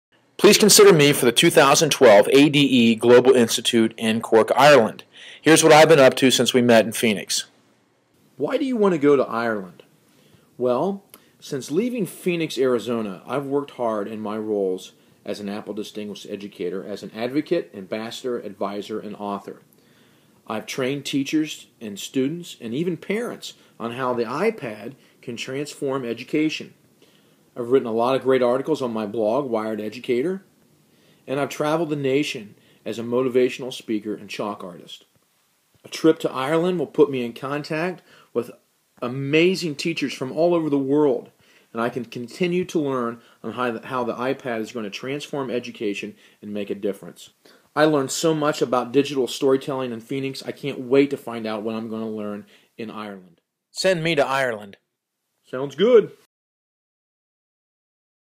Kelly Croy 2012 ADE Global Institute, Cork Ireland Application
Kelly Croy, educator, speaker, artist, and founder of Wired Educator, submits his application for the Ireland 2012 Global Institute. I wanted this to stand out so... I used Puppet Pals.  Made everything on my iPad.